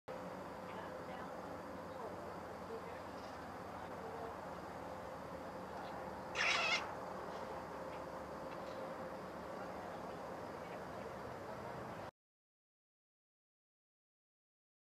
Squirrel Takes a Seat to Eat- PetTube
While perched in his chair, this cute squirrel eats some corn of the cob.